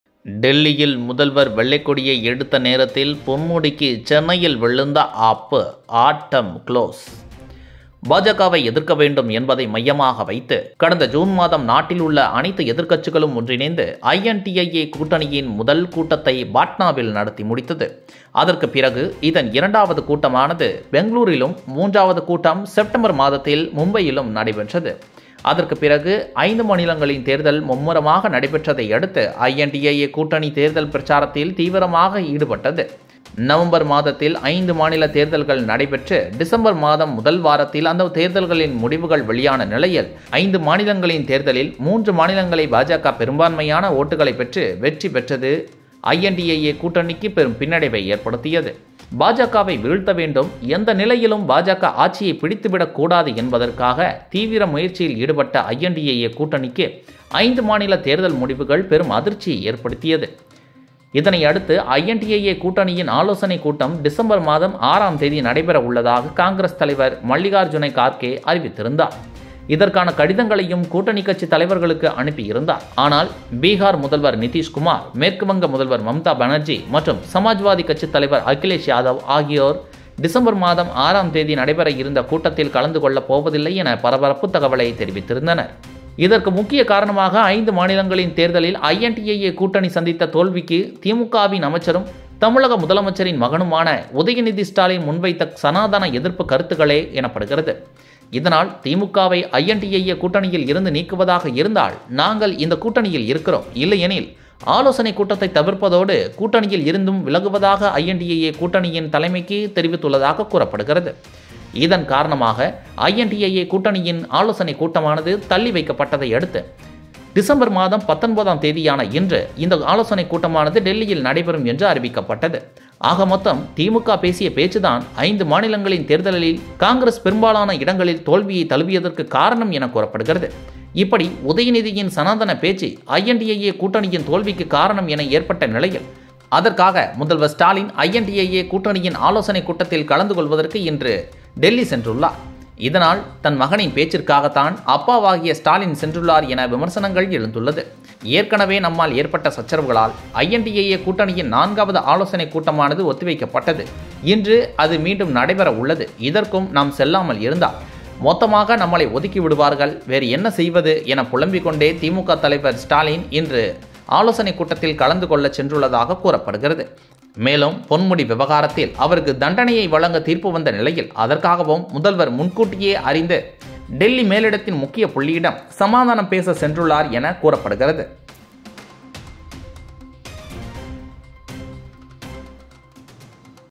டெல்லியில் முதல்வர் வெள்ளைக்கொடியை எடுத்த நேரத்தில் பொன்முடிக்கு சென்னையில் விழுந்த ஆப்பு... ஆட்டம்
டெல்லியில் முதல்வர் வெள்ளைக்கொடியை எடுத்த நேரத்தில் பொன்முடிக்கு சென்னையில் விழுந்த ஆப்பு... ஆட்டம் குளோஸ்.